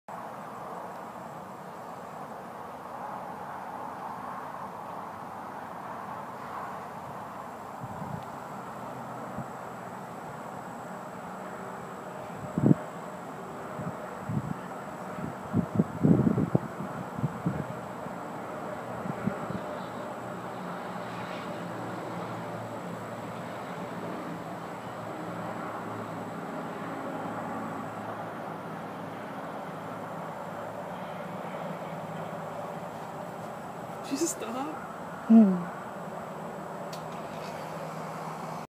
talia looking at an iphone 4s in the backyard
talia looking at an iphone 4s in the backyard.  It was her first time ever using an iphone.